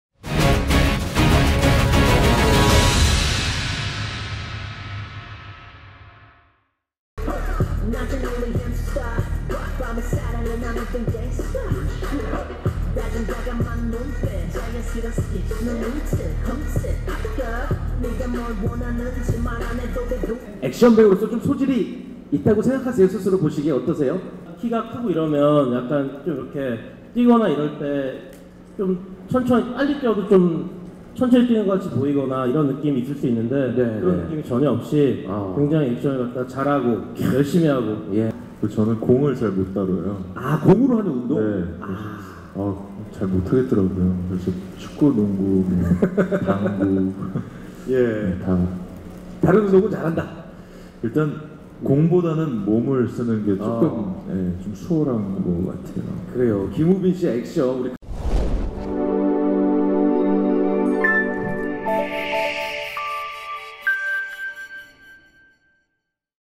[TF영상] '기술자들' 김우빈, "공보다는 몸으로 하는 일이 수월해"
[더팩트│조재형 기자] 영화 '기술자들' 쇼케이스가 8일 오후 서울 송파구 잠실 롯데시네마 월드타워점에서 열렸다. '기술자들'은 천재적 금고털이이자 작전 설계부터 모든 위조에 능한 멀티플레이어 지혁(김우빈 분)과 최연소 해커 종배(이현우 분)가 활약을 지켜보던 재계의 검은 손 조사장(김영철 분)이 둘을 끌어들이며 시작되는 '심장 쫄깃한 기술 한판'을 보여주는 영화다.

김홍선 감독이 메가폰을 잡고 배우 김우빈, 김영철, 고창석, 이현우, 조윤희, 신구, 임주환, 조달환 등이 출연하는 '기술자들'은 오는 24일 개봉한다.